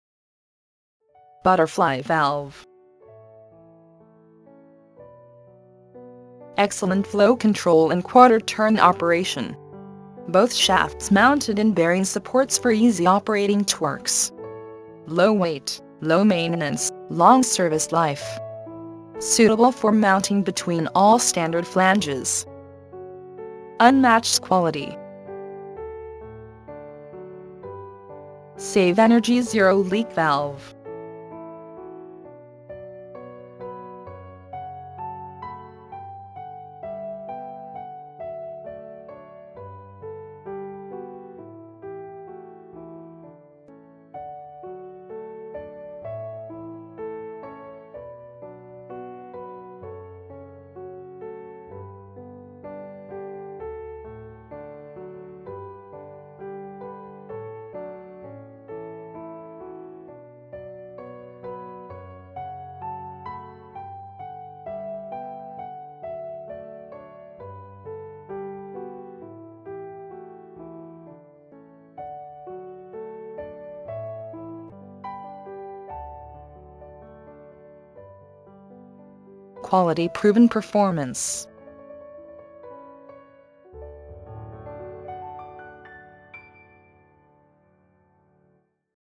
Butterfly Valve working video
"HP" make butterfly valve body liner which functions as the soft seat comes in an integrally molded (bonded) version and offers 100% bi-directional sealing against vacuum to rated pressures.

The wafer style body has universal design to fit between pipe flanges of almost all popular flange standards.

"HP" Have large range in Butterfly around 80 which includes Lug Wafer Type, Flanged type, Metal Seated, Triple Offset Design & Many more...